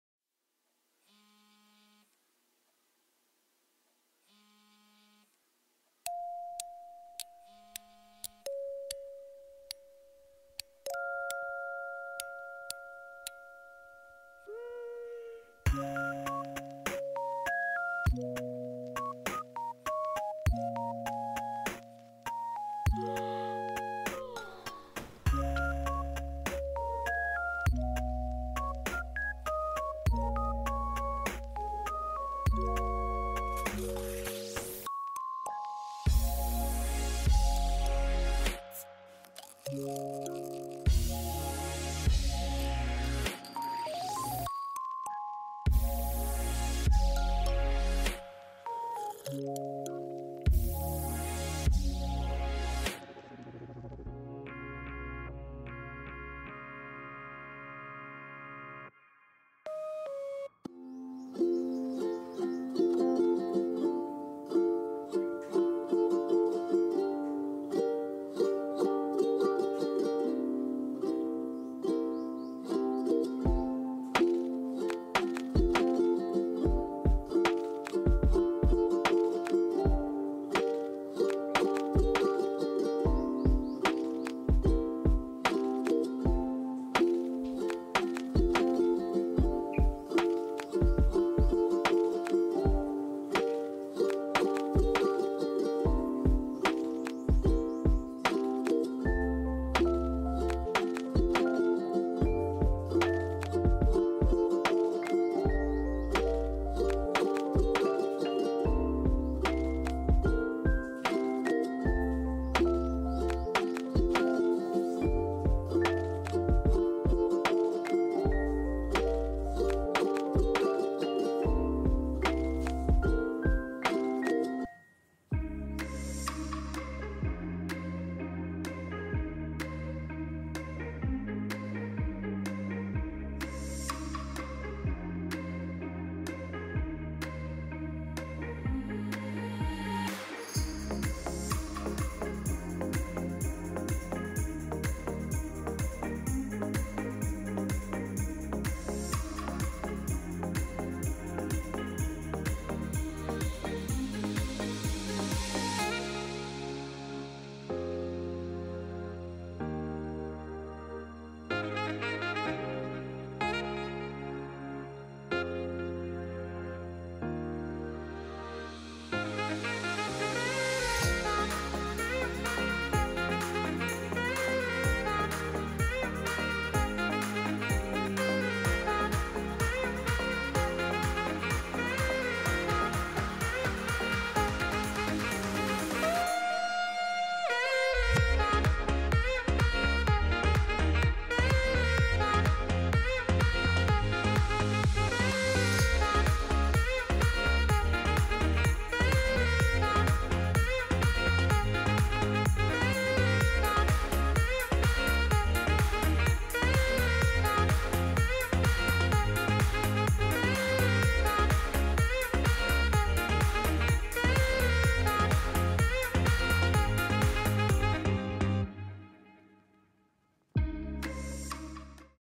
Introducing Yabi
Vlog 3 Hello All, just wanted to share a video of this beautiful gem. To me might be the most adorable baby of #2020
Introducing Yabi the beautiful baby

Musician: Rook1e

Musician: LiQWYD